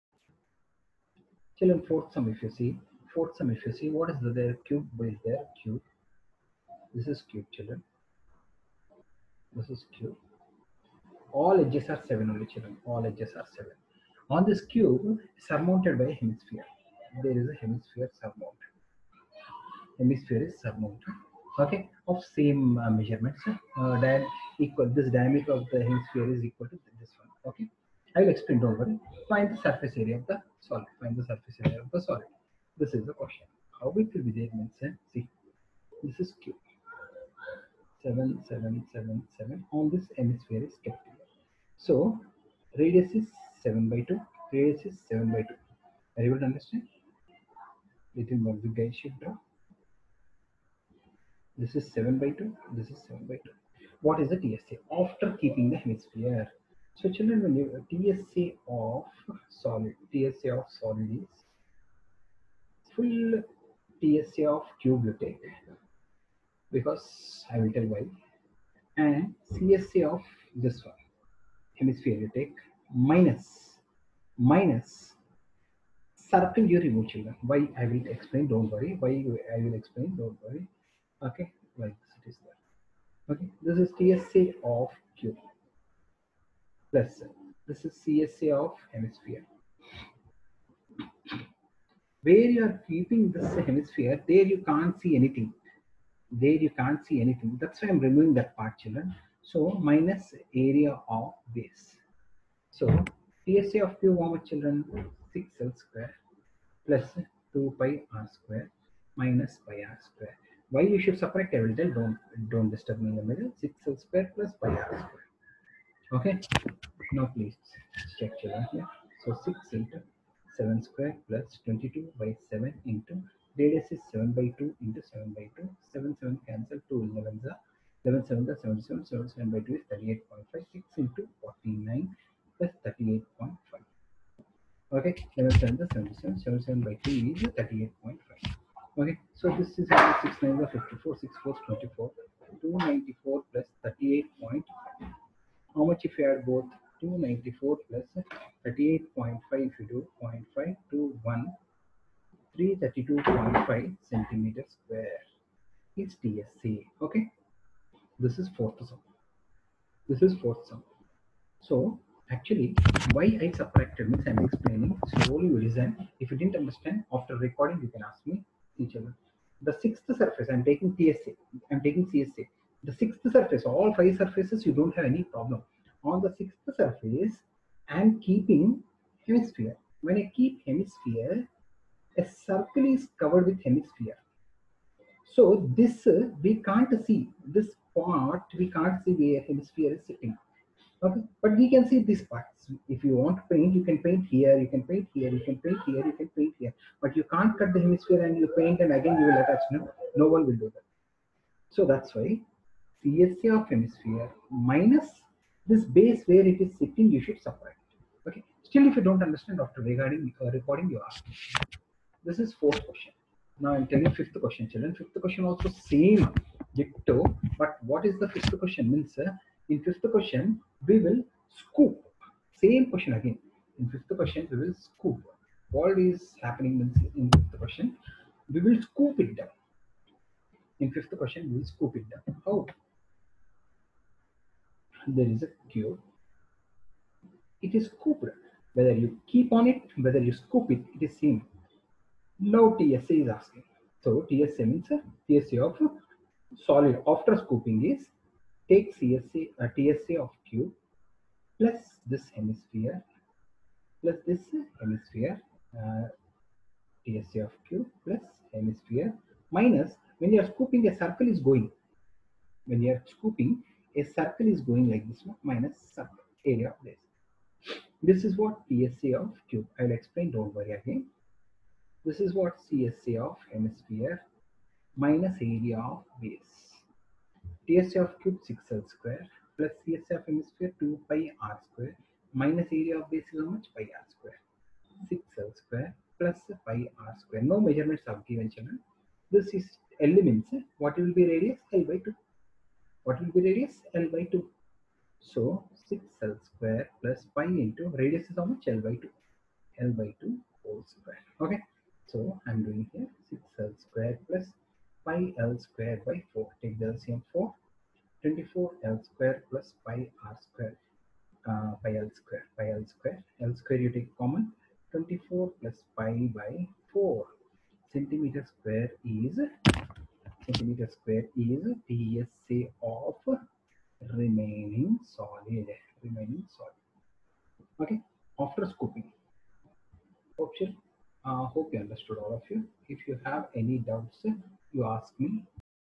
13.1 4th and 5th ncert maths 10th cbse
13.1 4th and 5th sum cbse 10th ncert maths text book.
A cubical block of side 7 cm is surmounted by a hemisphere of diameter equal to the side of the cube.Find the surface area of the solid.